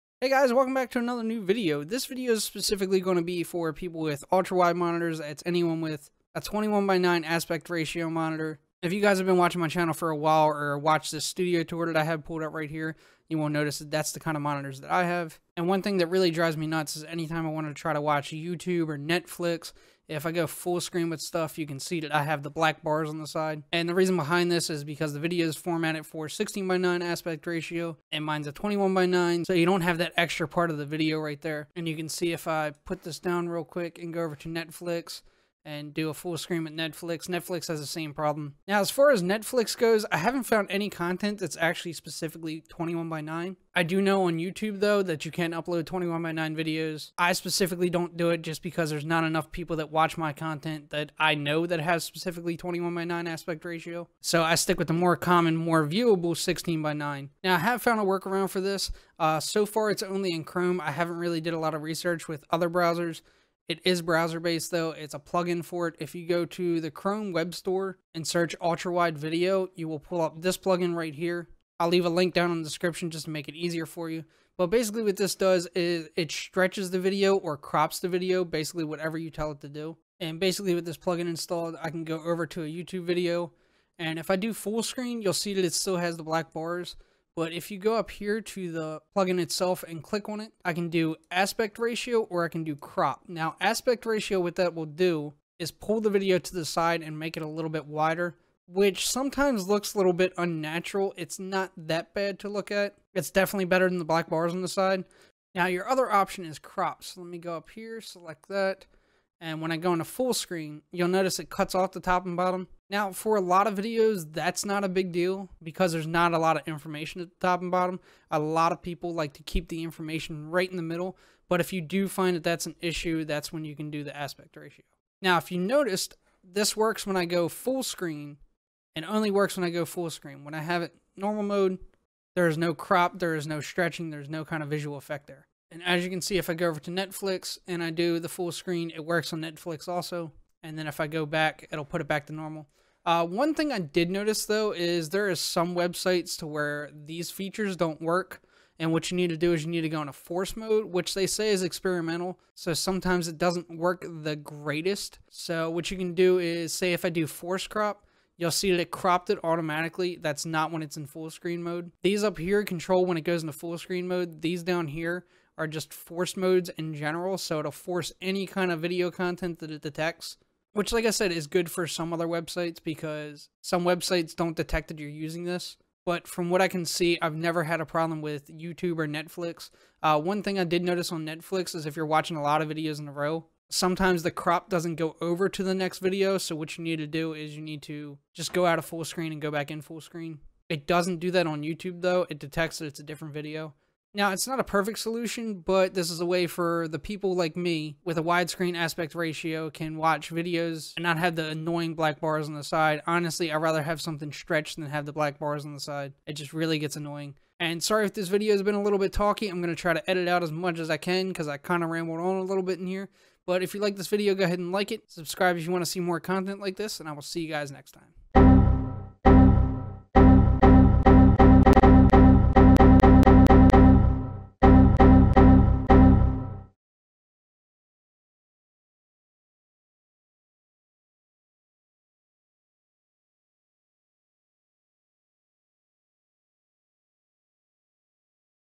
Get rid of black bars on Ultrawide Monitors
I thought this was a pretty cool concept so i figured i would share.